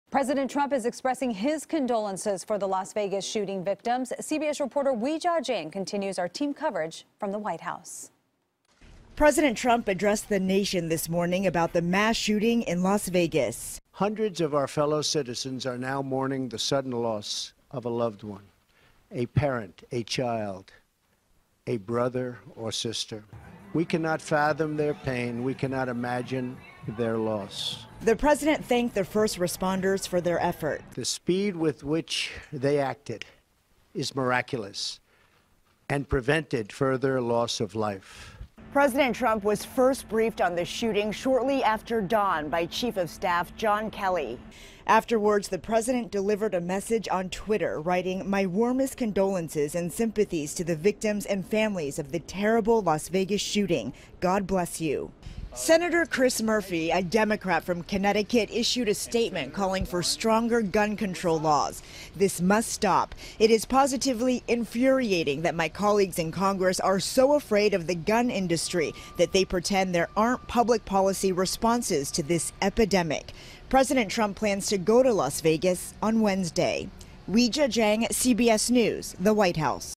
President Trump Offers Condolences After Vegas Mass Shooting
Weijia Jiang reports on President's address in wake of deadly Las Vegas Strip concert shooting (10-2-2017)